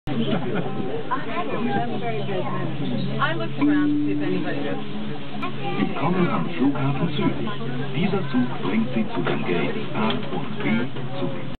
チューリッヒ空港 スカイトレイン
スイスのチューリッヒ空港。長距離線が発着する「ドックＥ」から、ヨーロッパ域内への乗り継ぎのため「ドックＡ」へ移動するには、スカイトレインに乗ります。ガラス張りの綺麗なケーブルカーです。